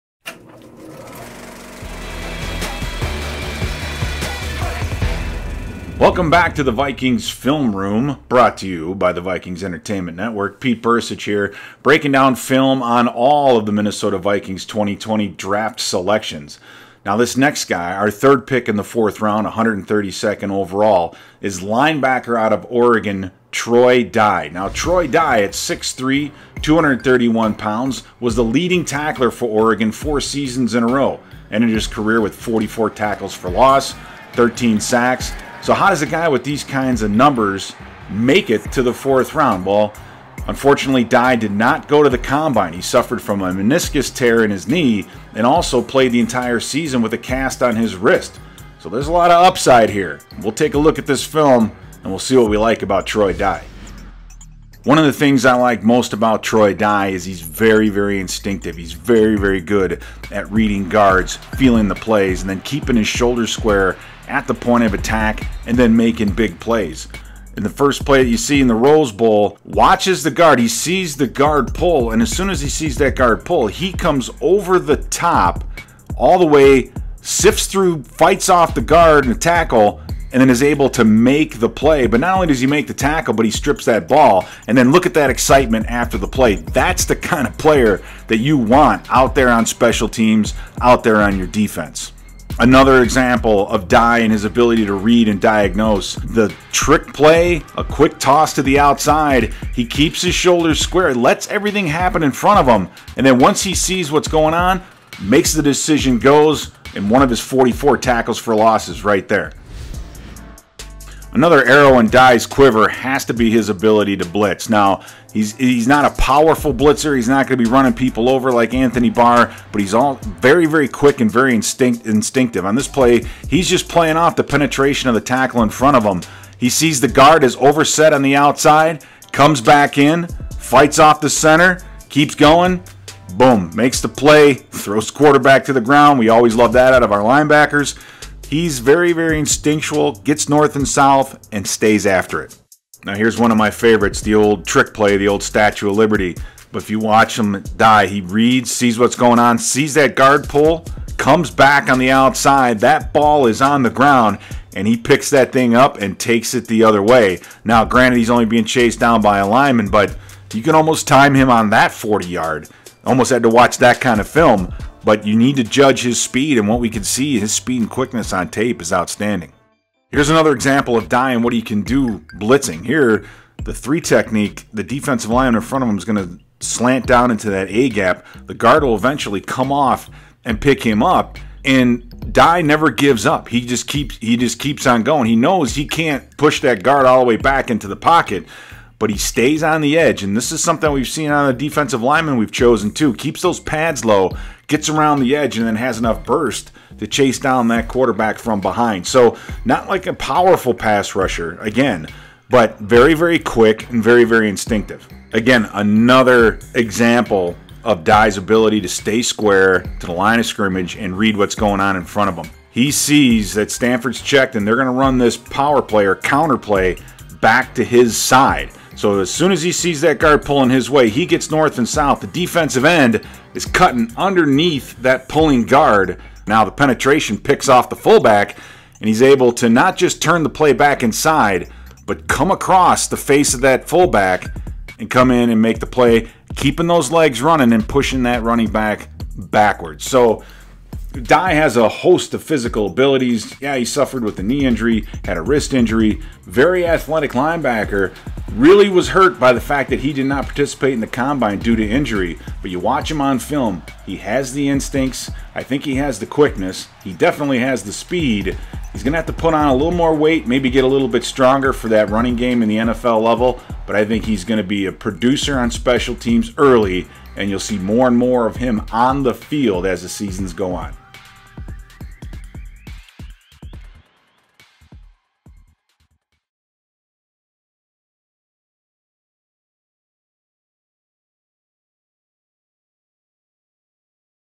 Film Room: Game Tape Shows LB Troy Dye's Instincts and Upside | Minnesota Vikings
Former Minnesota Vikings player and coach Pete Bercich continues his breakdown of the team's 15-man 2020 draft class by looking at LB Troy Dye, a fourth round selection out of Oregon and how he could make an impact on special teams early in his career, while developing into a defensive starter down the road.

AP Photo/TonyAvelar